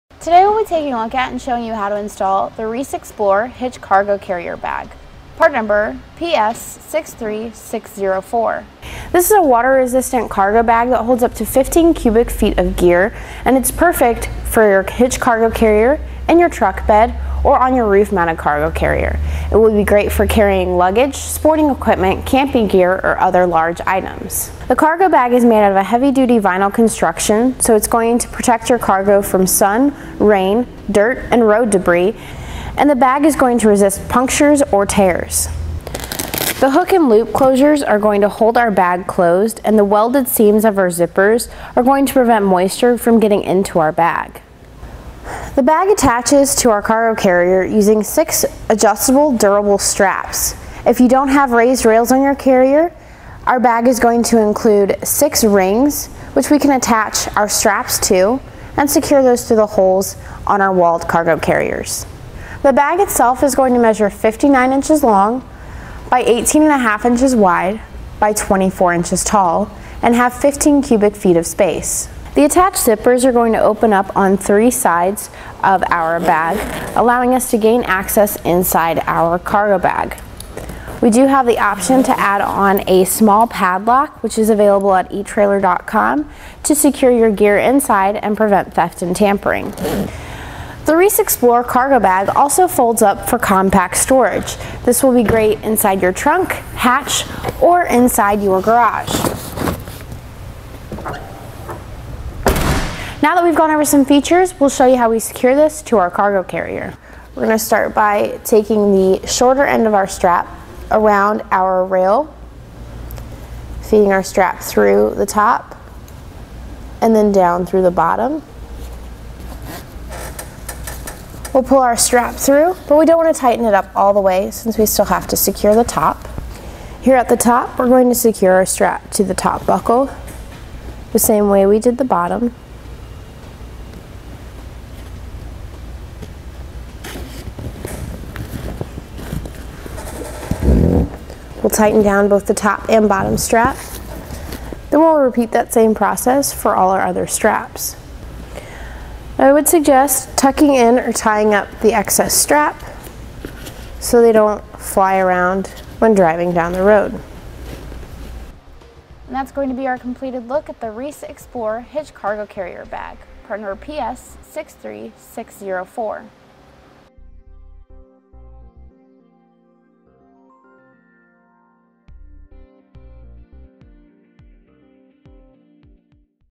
etrailer | An In-Depth Look at the Reese Amigo Bag for Hitch-Mounted Cargo Carrier
Click for more info and reviews of this Reese Hitch Cargo Carrier Bag:

Speaker 1:          Today, we'll be taking a look at and showing you to install, the Reese Explore Hitch Cargo Carrier bag, part number: PS63604.This is a water-resistant cargo bag that holds up to 15 cubic feet of gear and it's perfect for your hitch cargo carrier in your truck bed or on your roof-mounted cargo carrier. It would be great for carrying luggage sporting equipment, camping gear or other large items.The cargo bag is made out of a heavy-duty vinyl construction, so it's going to protect your cargo from sun, rain, dirt and road debris; and the bag is going to resist punctures or tears. The hook-and-loop closures are going to hold our bag closed, and the welded seams of our zippers are going to prevent moisture from getting into our bag.The bag attaches to our cargo carrier using six adjustable, durable straps. If you don't have raised rails on your carrier, our bag is going to include six rings, which we can attach our straps to and secure those to the holes on our walled cargo carriers.The bag itself is going to measure 59 inches long by 18-and-a-half inches wide by 24 inches tall and have 15 cubic feet of space. The attached zippers are going to open up on three sides of our bag, allowing us to gain access inside our cargo bag. We do have the option to add on a small padlock, which is available at etrailer.com, to secure your gear inside and prevent theft and tampering.The Reese Explore Cargo Bag also folds up for compact storage.

This will be great inside your trunk, hatch or inside your garage. Now, that we've gone over some features, we'll show you how we secure this to our cargo carrier.We're going to start by taking the shorter end of our strap around our rail, feeding our strap through the top and then down through the bottom. We'll pull our strap through, but we don't want to tighten it up all the way since we still have to secure the top.Here at the top, we're going to secure our strap to the top buckle the same way we did the bottom. We'll tighten down both the top and bottom strap, then we'll repeat that same process for all our other straps. I would suggest tucking in or tying up the excess strap so they don't fly around when driving down the road.That's going to be our completed look at the Reese Explorer Hitch Cargo Carrier Bag, part number: PS63604..